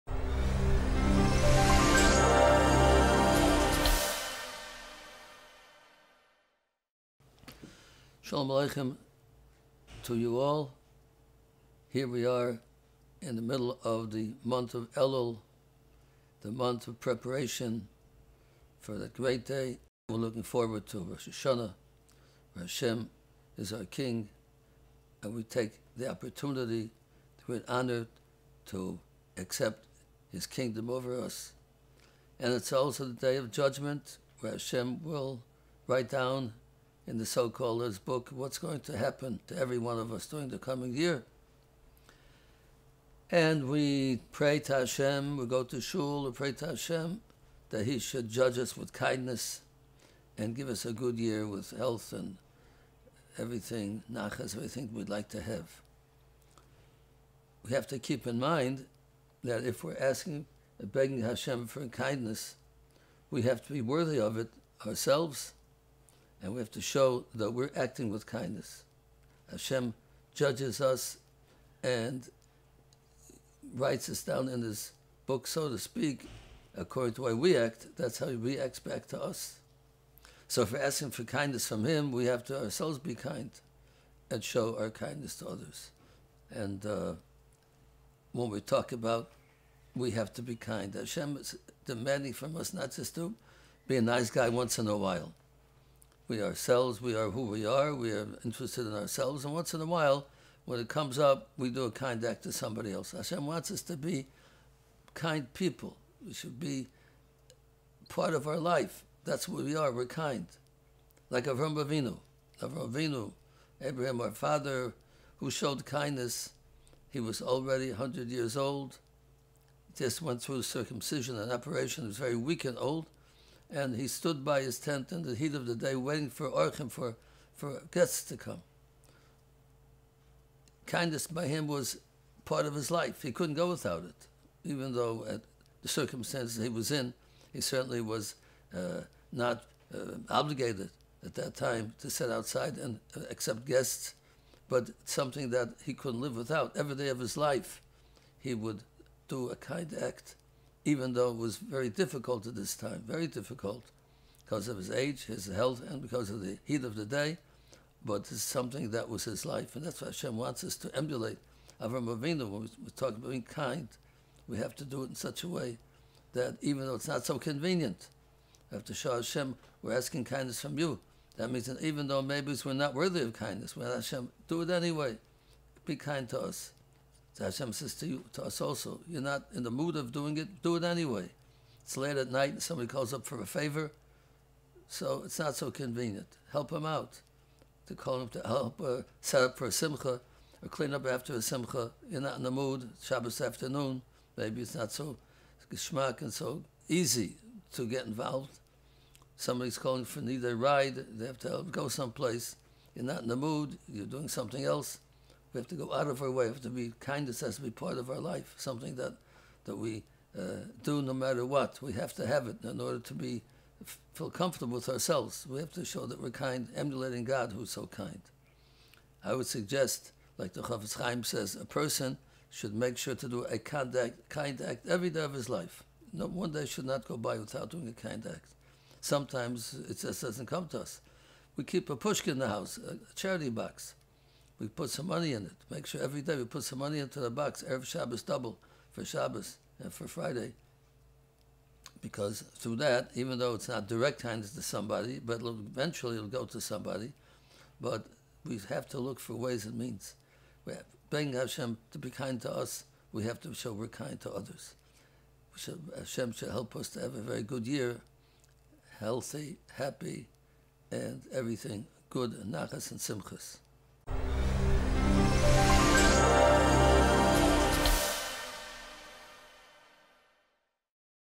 Rosh Hashana with Meaning: Be Kind | Rabbi Chaim Mintz
How can we ensure a good judgment for the coming year? Oorah's founder, Rabbi Chaim Mintz delivers a short Rosh Hashana message for Oorah's families. You can watch Rabbi Mintz's weekly class given each Tuesday night at 8:30pm.